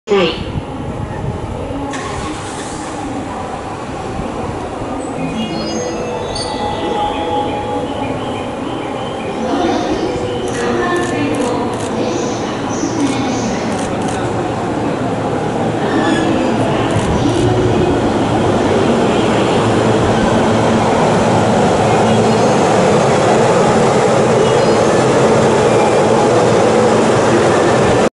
「E231-500に記念HM！」E231系500番台 八ミツ A511編成 JR東日本 中央・総武線 荻窪駅 到着
10月10日撮影。
投稿がかなり遅れてしまいすみません
現在、中央・総武線では「中野電車区開設100周年記念」としてE231系2編成にHMが掲出されていて今回はE231-500のHMです。デザインもかっこよく、よく似合ったHMだと思いました。